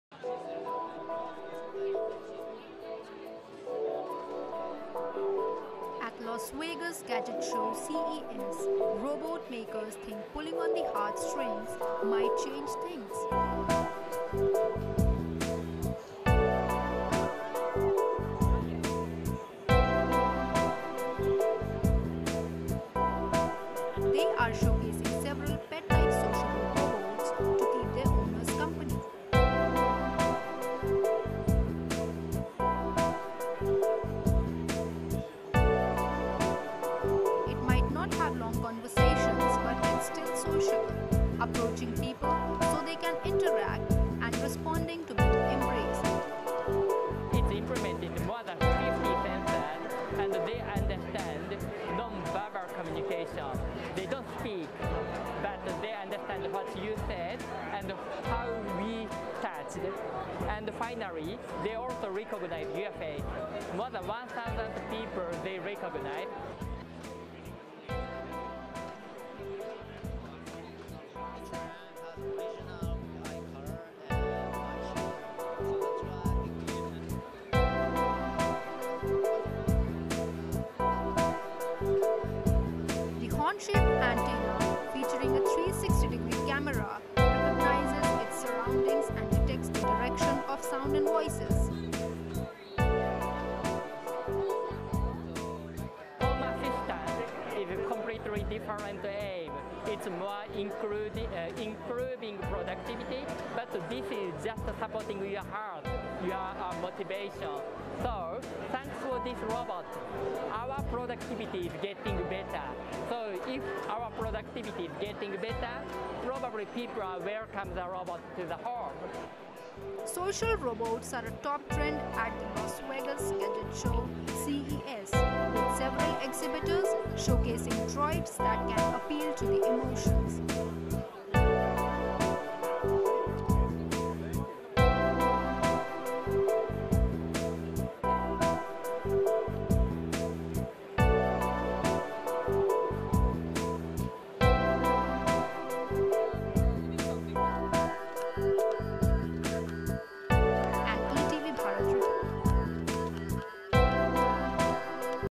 Cute robots to be future companions for humans
Las Vegas: Robots might be widely used in industry, but they're yet to make the journey to many consumers' homes. At Las Vegas gadget show CES, robot makers think pulling on the heart strings might change things. They're showcasing several pet-like, sociable robots to keep their owners' company.